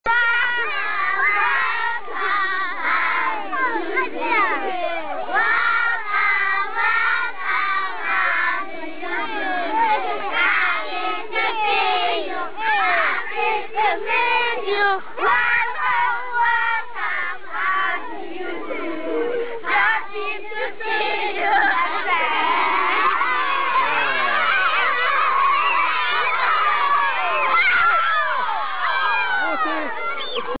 Welcome song in Sogakope
This song is used to welcome visitors. At the end of it the childrens storm to the visitor. This time they overran me.